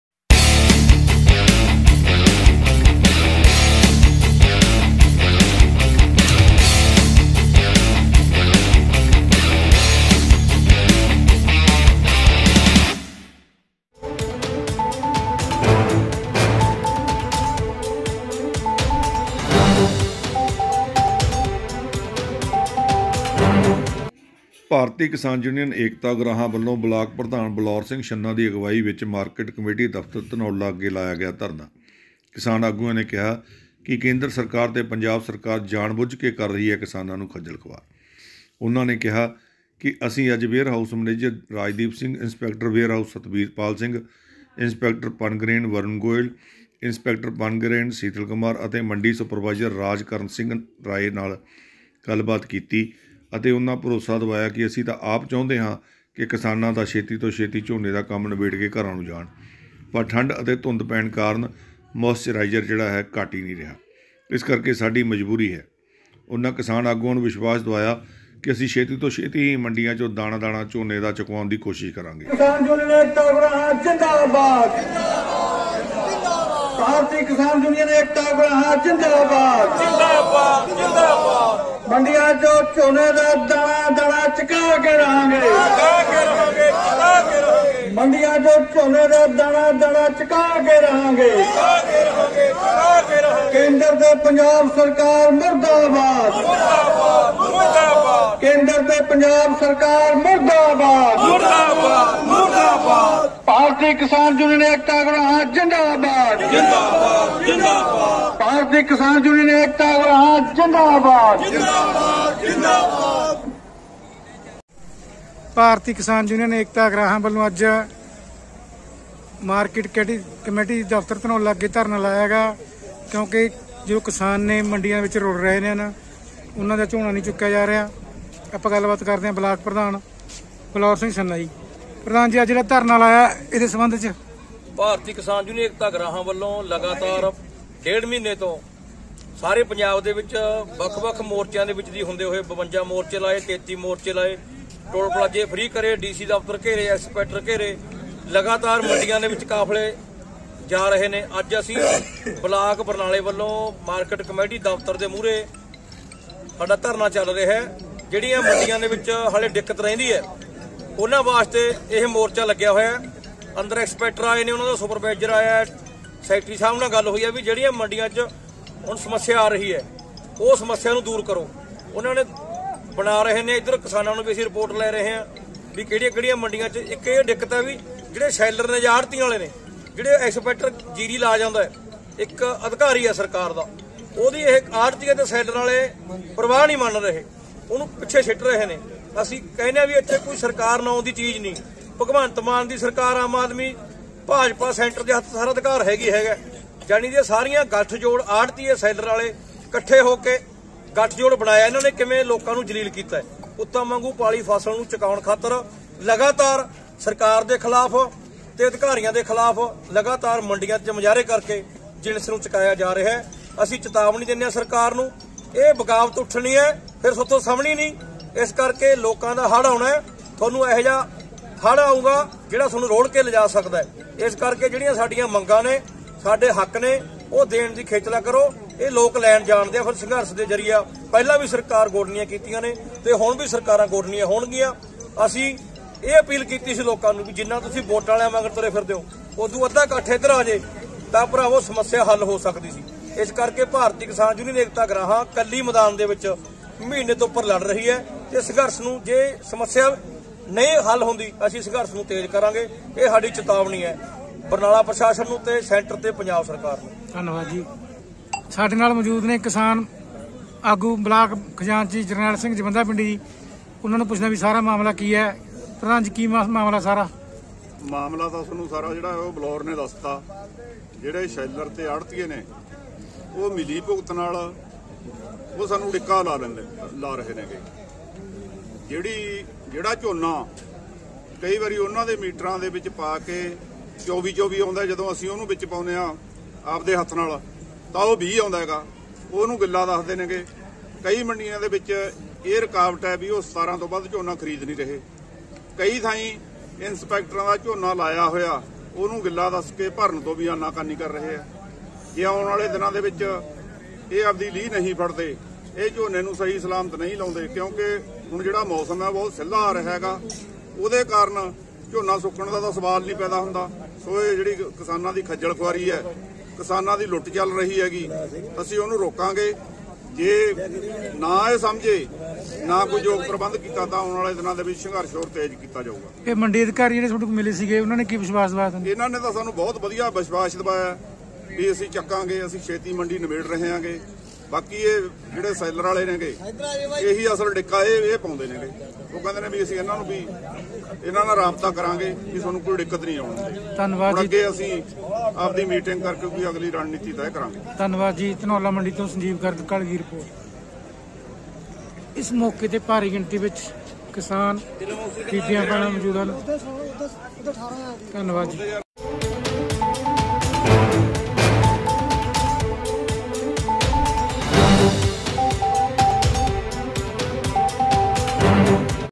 @BBCINDIANEWSTVਕਿਸ ਨੇ ਕਿਹਾ ਕੇਂਦਰ ਸਰਕਾਰ ਤੇ ਪੰਜਾਬ ਸਰਕਾਰ ਜਾਣ ਬੁਝ ਕੇ ਕਿਸਾਨਾਂ ਨੂੰ ਖੱਜਲ ਖੁਆਰ ਕਰ ਰਹੀ ਹੈ।
@BBCINDIANEWSTV ਭਾਰਤੀ ਕਿਸਾਨ ਯੂਨੀਅਨ ਏਕਤਾ ਉਗਰਾਹਾਂ ਵੱਲੋਂ ਬਲਾਕ ਪ੍ਰਧਾਨ ਬਲੌਰ ਸਿੰਘ ਛੰਨਾ ਦੀ ਅਗਵਾਈ ਵਿੱਚ ਮਾਰਕੀਟ ਕਮੇਟੀ ਦਫਤਰ ਧਨੌਲਾ ਅੱਗੇ ਲਾਇਆ ਗਿਆ ਧਰਨਾ।
ਕਿਸਾਨ ਆਗੂਆਂ ਨੇ ਕਿਹਾ ਕਿ ਕੇਂਦਰ ਸਰਕਾਰ ਤੇ ਪੰਜਾਬ ਸਰਕਾਰ ਜਾਣ ਬੁਝ ਕੇ ਕਿਸਾਨਾਂ ਨੂੰ ਖੱਜਲ ਖੁਆਰ ਕਰ ਰਹੀ ਹੈ।

ਉਹਨਾਂ ਕਿਹਾ ਕਿ ਅੱਜ ਅਸੀਂ ਵੇਅਰ ਹਾਊਸ ਮੈਨੇਜਰ ਰਾਜਦੀਪ ਸਿੰਘ, ਇੰਸਪੈਕਟਰ ਵੇਅਰਹਾਊਸ ਸਤਬੀਰ ਪਾਲ ਸਿੰਘ, ਇੰਸਪੈਕਟਰ ਪਰਨਗਰੇਨ ਵਰੁਨ ਗੋਇਲ, ਇੰਸਪੈਕਟਰ ਪਨਗਰੇਨ ਸੀਤਲ ਕੁਮਾਰ, ਅਤੇ ਮੰਡੀ ਸੁਪਰਵਾਈਜ਼ਰ ਰਾਜਕਰਨ ਸਿੰਘ ਰਾਏ ਹੋਰਾਂ ਨਾਲ ਗੱਲਬਾਤ ਕੀਤੀ ਅਤੇ ਉਨਾਂ ਭਰੋਸਾ ਦਿਵਾਇਆ ਕਿ ਅਸੀਂ ਤਾਂ ਆਪ ਚਾਹੁੰਦੇ ਹਾਂ ਕਿ ਕਿਸਾਨ ਛੇਤੀ ਤੋਂ ਛੇਤੀ ਆਪਣਾ ਝੋਨੇ ਦਾ ਕੰਮ ਨਿਬੇੜ ਕੇ ਘਰਾਂ ਨੂੰ ਜਾਣ ਪ੍ਰੰਤੂ ਠੰਡ ਅਤੇ ਧੁੰਦ ਪੈਣ ਕਾਰਨ ਮੋਇਸਚਰਾਈਜ਼ ਘੱਟ ਨਹੀਂ ਰਿਹਾ ਇਸ ਕਰਕੇ ਸਾਡੀ ਮਜਬੂਰੀ ਹੈ ਉਹਨਾਂ ਕਿਸਾਨ ਆਗੂਆਂ ਨੂੰ ਵਿਸ਼ਵਾਸ ਦਿਵਾਇਆ ਕਿ ਅਸੀਂ ਛੇਤੀ ਤੋਂ ਛੇਤੀ ਹੀ  ਮੰਡੀਆਂ ਚੋਂ ਦਾਣਾ ਦਾਣਾ ਝੋਨੇ ਦਾ ਚੁਕਵਾਉਣ ਦੀ ਕੋਸ਼ਿਸ਼ ਕਰਾਂਗੇ।
ਇਸ ਮੌਕੇ ਤੇ ਜਰਨੈਲ ਸਿੰਘ ਜਵੰਦਾ ਪਿੰਡੀ ਨਿਰਪਜੀਤ ਸਿੰਘ ਬਡਬਰ, ਕਰਿਸ਼ਨ ਸਿੰਘ ਛੰਨਾ ,ਬਲਵਿੰਦਰ ਸਿੰਘ ਛੰਨਾ, ਕੇਵਲ ਸਿੰਘ ਧਨੌਲਾ, ਦਰਸ਼ਨ ਸਿੰਘ ਹਰੀਗੜ੍ਹ, ਭਗ barnala loksabhaelection aamaadmiparty pmmodi aap naredramodi dr babby bubby pmmodilive ਤ ਸਿੰਘ ਛੰਨਾ, ਦਰਸ਼ਨ ਸਿੰਘ ਭੈਣੀ ਤੋਂ ਇਲਾਵਾ ਭਾਰੀ ਗਿਣਤੀ ਵਿੱਚ ਕਿਸਾਨ ਮਹਿਲਾਵਾਂ ਮੌਜੂਦ ਸਨ।

ਧਨੋਲਾ ਮੰਡੀ ਤੋਂ ਸੰਜੀਵ ਕਾਲੀ ਦੀ ਰਿਪੋਰਟ